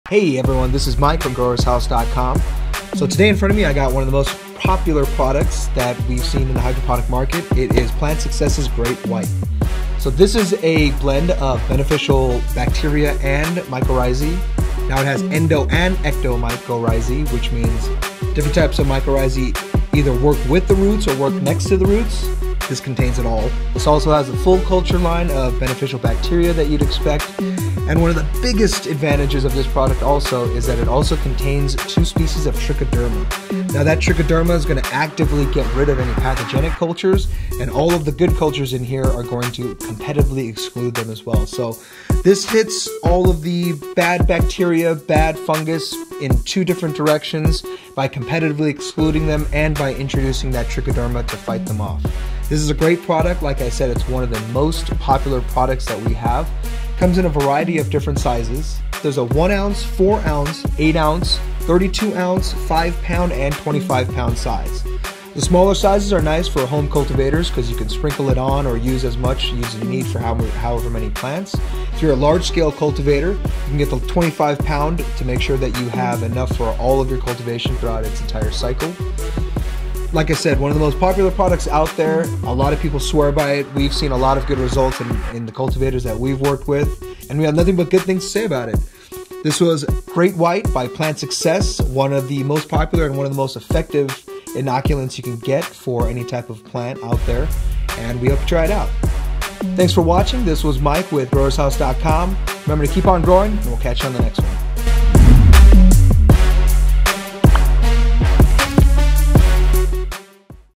GreatWhite Mycorrhizae
DESCRIPTION
Great White is the most advanced mycorrhizal product on the market today. The combination of mycorrhizae, beneficial bacteria, trichoderma, and plant vitamins will give your plants a strong and developed root system. This will enable your plants to break down and absorb nutrients efficiently and effectively. Also, it will increase water uptake and the overall absorption area of the root system, resulting in a healthier plant. Can be used in hydroponics as well as soil. May also be injected in to media.

This cutting edge formula contains 15 different species of mycorrhizal fungi, 11 different species of beneficial bacteria and 2 species of trichoderma all in one product. Research shows that this powerful formula will ignite plant and root growth giving plants the tools they need to maximize yields. Great White's concentrated formula ensures optimum colonization of root systems by the fungi for less money. The water soluble powder makes application a snap and delivers the spores directly to the roots for immediate germination. Users should look for explosive root growth, increased yields, increased fruiting and flowering, increased nutrient and water absorption and improved transplant success. The key to healthy and vital plants starts with a vigorous root system and a vigorous root system starts with Great White!